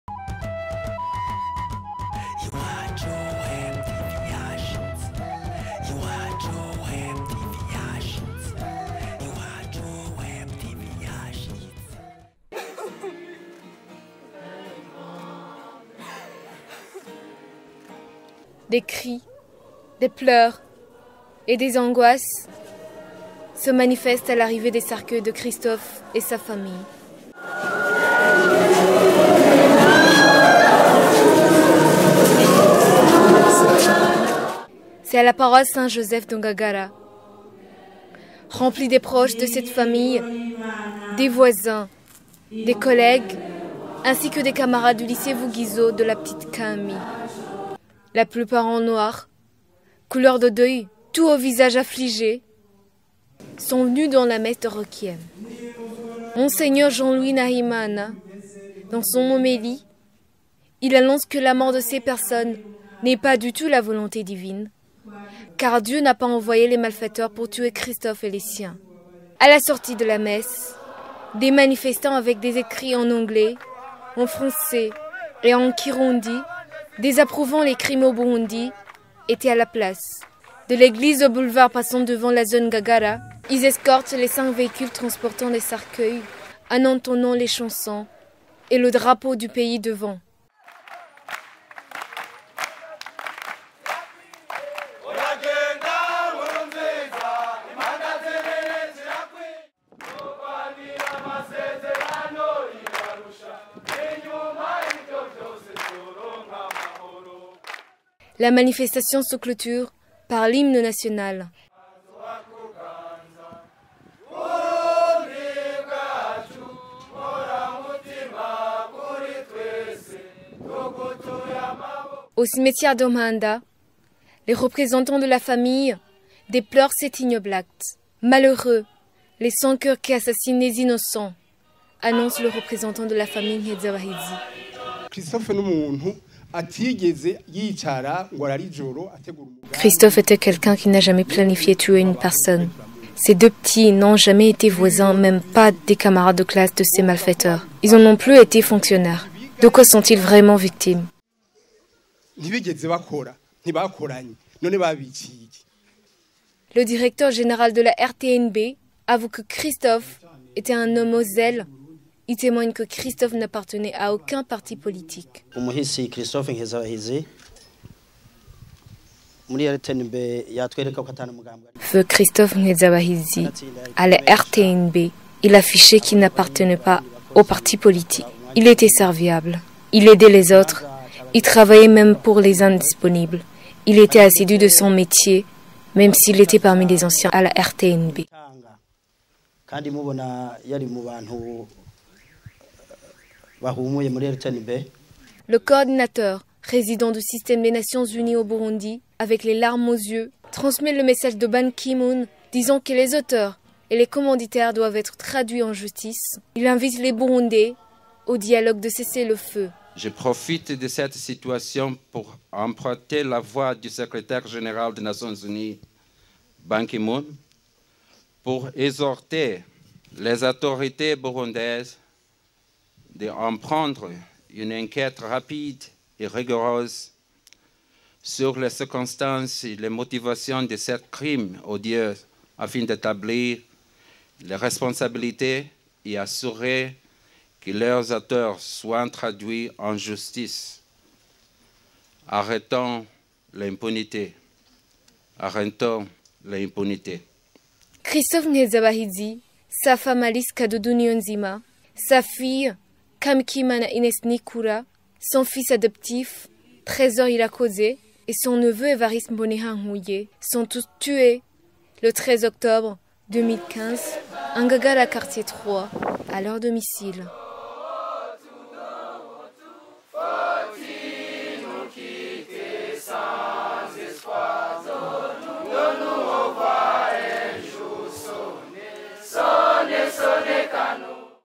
Les tristes Adieux à Christophe Nkezabahizi et quatre membres de sa famille
Avec tant de tristesse, Christophe de la RTNB  et sa famille enterrés ce mardi à Mpanda. Le représentant des Nations Unies au Burundi transmet le message de Ban Ki Moon, le secrétaire général des Nations Unies appellant les Burundais au dialogue.